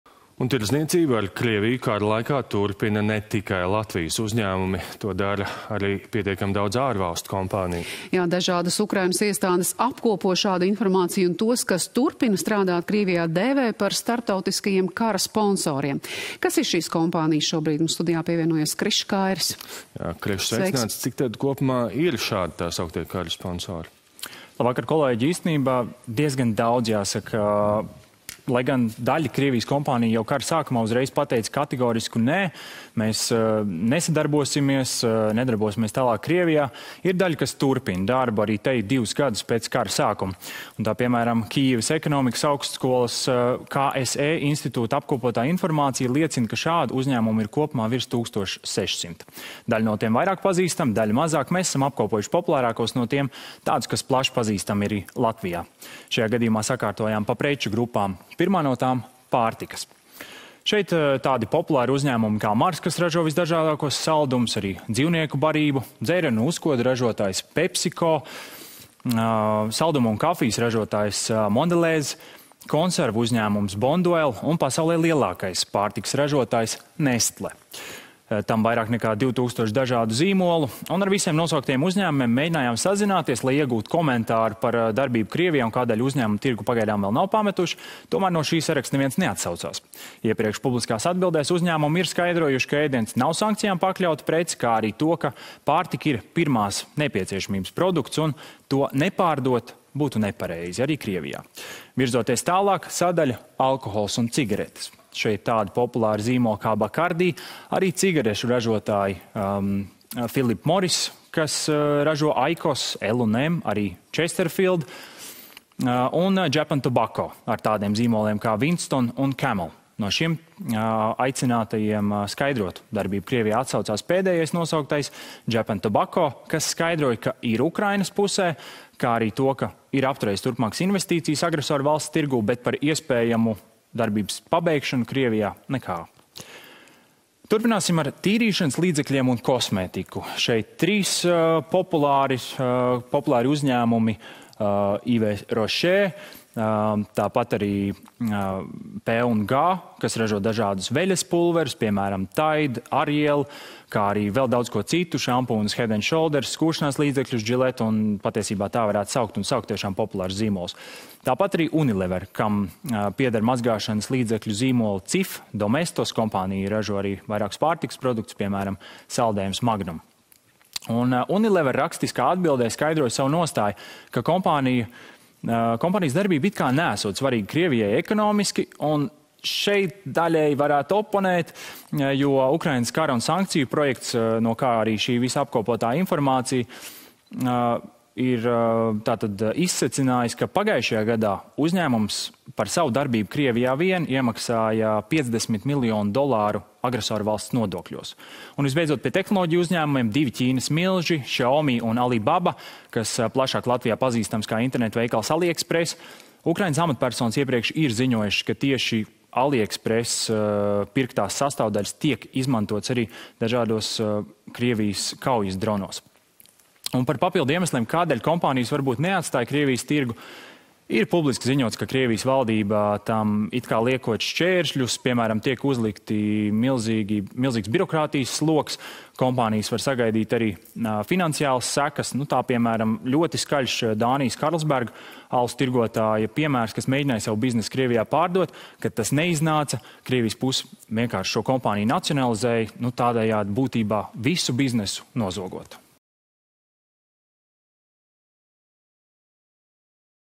Daudzi lielie uzņēmumi turpina darbu Krievijā
Tirdzniecību ar Krieviju kara laikā turpina ne tikai Latvijas uzņēmumi vien, arī pietiekami daudz ārvalstu kompāniju. Dažādas Ukrainas iestādes apkopo šādu informāciju, tos, kas turpina strādāt Krievijā, dēvējot par “starptautiskajiem kara sponsoriem”.